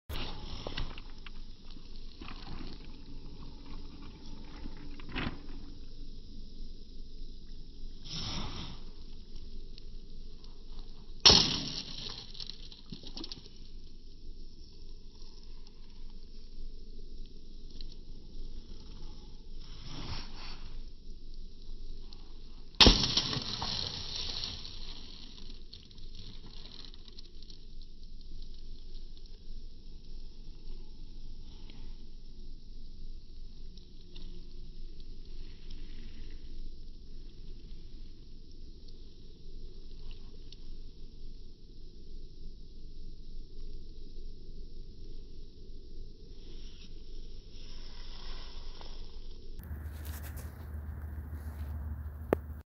Breaking ice with rocks in slow motion (11/25/2023)
Next to the tracks on the curve just east of the Lewistown crossovers is this spot where water tends to puddle in the ballast.